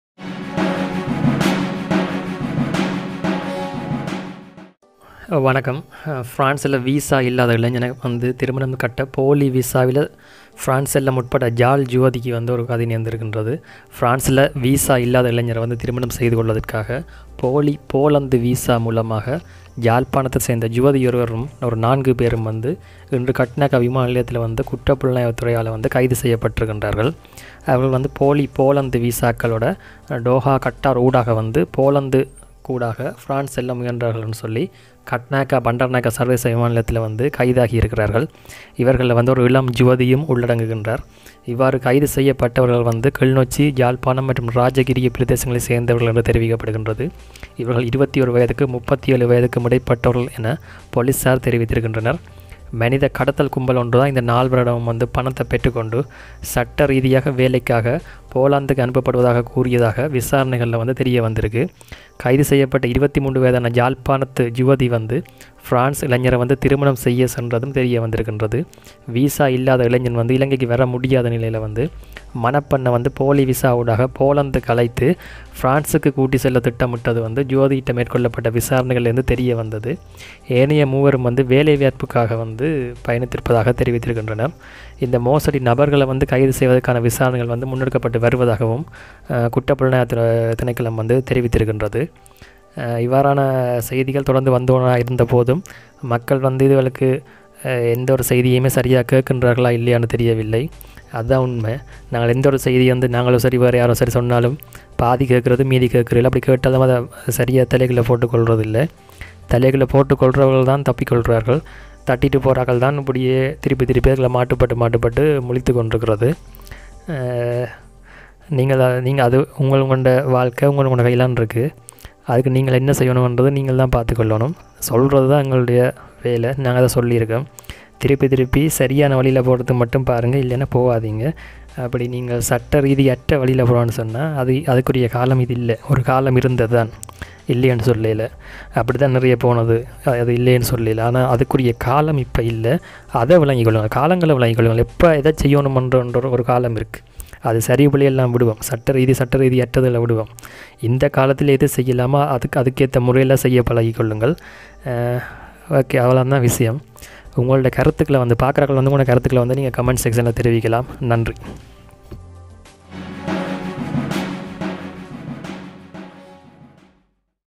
போலி பிரான்ஸ் திருமண வீசா | யாழ் யுவதிக்கு நேர்ந்த கதி | France Paris tamil Updates
France marriage visa scan news in tamil